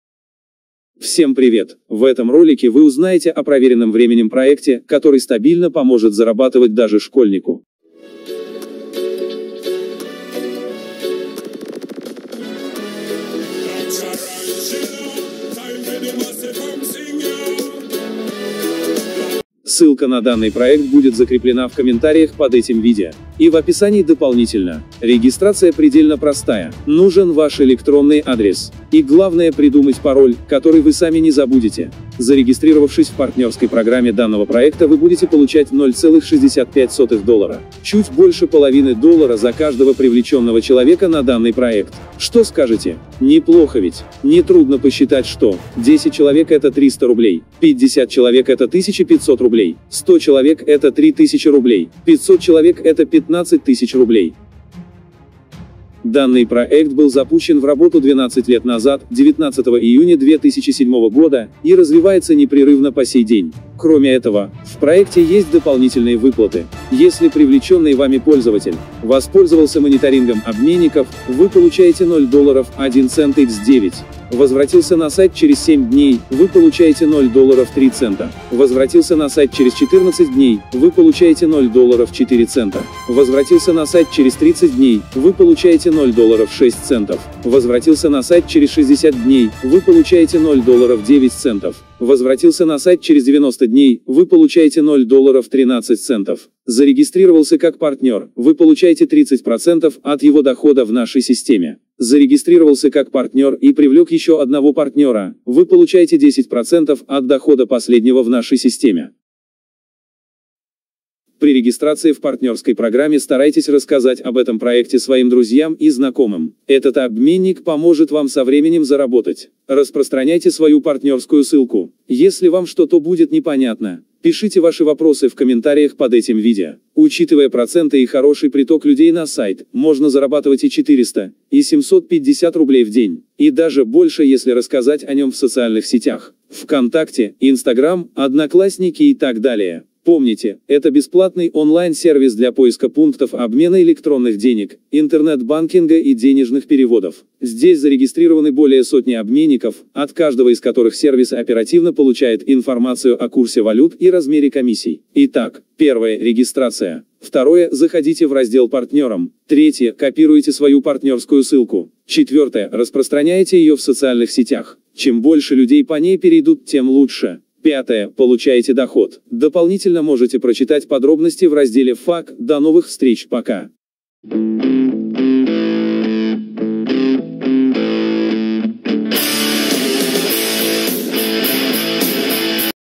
ЗАРАБОТОК В ИНТЕРНЕТЕ БЕЗ ВЛОЖЕНИЙ. КАК ЗАРАБОТАТЬ ДЕНЬГИ В ИНТЕРНЕТЕ. ЗАРАБОТОК НА АВТОМАТЕ.
*ЗАРАБОТОК В ИНТЕРНЕТЕ*(ПРОВЕРЕННЫЙ) -
СМОТРИ НЕ ПРОПУСТИ КРУТЫЕ ОБЗОРЫ:
Всем привет. В этом ролике вы узнаете о проверенном временем проекте, который стабильно поможет зарабатывать даже школьнику! Ссылка на
данный проект будет закреплена в комментариях под этим видео. И в описании дополнительно. Регистрация предельно простая. Нужен ваш электронный адрес.
И главное придумать пароль, который вы сами не забудете. Зарегистрировавшись в партнерской программе данного проекта вы будете получать 0.65 доллара.
Чуть больше половины доллара за каждого привлеченного человека на данный проект! Что скажете? Не плохо ведь. Не трудно посчитать что
10 человек это 300 рублей.
50 человек это 1500 рублей.
100 человек это 3000 рублей.
500 человек это 15 000 рублей.
Данный проект был запущен в работу 12 лет назад (19 июня 2007) и развивается непрерывно по сей день. Кроме этого, в проекте есть дополнительные
 зарегистрировался как партнер и привлек еще одного партнера, вы получаете 10% от дохода последнего в нашей системе.
При регистрации в партнерской программе старайтесь рассказать об этом проекте своим друзьям и знакомым. Этот обменник поможет вам со временем
заработать. Распространяйте свою партнерскую ссылку. Если вам что то будет не понятно - пишите ваши вопросы в комментариях под этим видео. Учитывая
проценты и хороший приток людей на сайт, можно зарабатывать и 400, и 750 рублей в день. Вконтакте, Инстаграм, Одноклассники и так далее. Помните, это бесплатный онлайн-сервис для поиска пунктов обмена электронных денег, интернет-банкинга и денежных переводов. Здесь зарегистрированы более сотни обменников, от каждого из которых сервис оперативно получает информацию о курсе валют и размере комиссий.
Итак. Первое - регистрация. Второе - заходите в раздел партнерам. Третье - копируете свою партнерскую ссылку. Четвертое - распространяете ее в социальных
сетях. Чем больше людей по ней перейдут тем лучше. Пятое - получаете доход. Дополнительно можете прочитать подробности в разделе FAQ. До новых встреч,
пока.